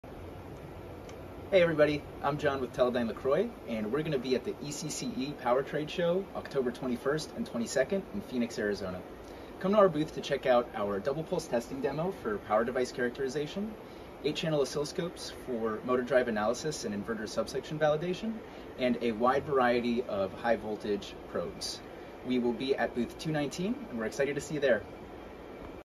Teledyne LeCroy at ECCE 2024
My name is Jon Shecter and I’m excited to announce that we’ll be participating in the ECCE Power Trade Show on October 21st and 22nd in Phoenix, Arizona. I invite you to visit our booth, where we’ll be showcasing a double pulse testing demo for power device characterization, eight-channel oscilloscopes for motor drive analysis, and high voltage probes. Our team is eager to engage with you at booth 219.